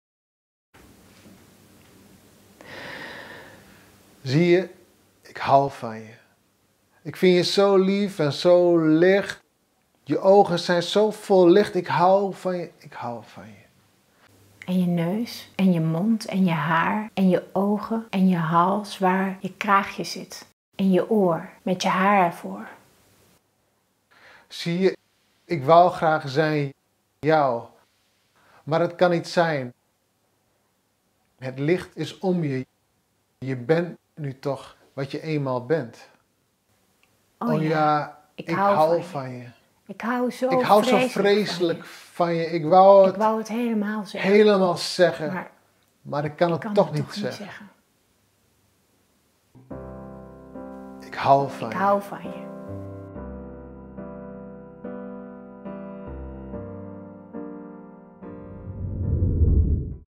Zie je, ik hou van je - Valentijnsdagteaser van de KB
Valentijnsdagteaser van de KB

Vandaag begint de poëzieweek met het thema 'samen'! In het kader hiervan organiseren we op Valentijnsdag een live KB-Café: Geef me de liefde!

Hoe zit de liefde door de eeuwen heen vervlochten door de Nederlandse literatuur en dichtkunst? Dit thema wordt besproken tijdens het webinar, mét de favoriete gedichten voorgedragen door Tsead Bruinja Heidi Koren en Anne Vegter.

FIlmproductie door: Frank Kouws